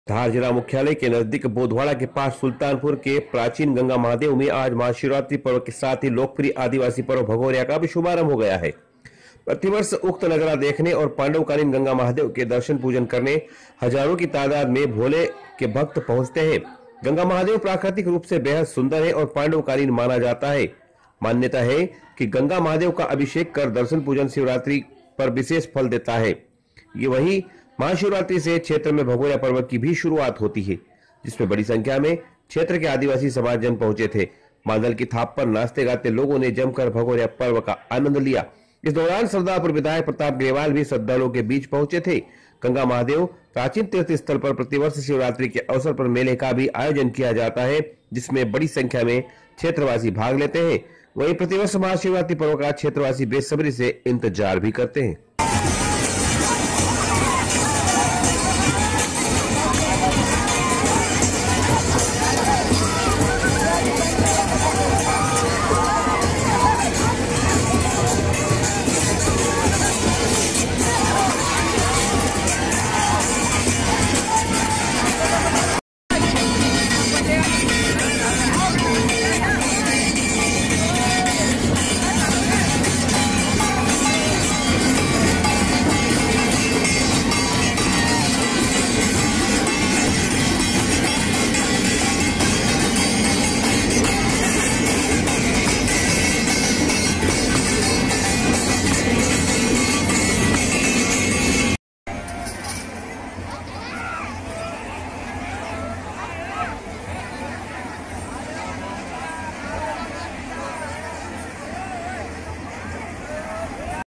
Ganga Mahdev Bhagoriya Aagaj - DHAR NEWS CHANNEL
धार - लोकप्रिय आदिवासी पर्व भगोरिया का महाशिवरात्रि से हुआ आगाज , प्राकृतिक सुंदरता से घिरे महाभारत कालीन गंगा महादेव में दर्शनों के लिए हजारों भक्त पहुचे ,  पूरी खबर देखें धार न्यूज़ चैनल पर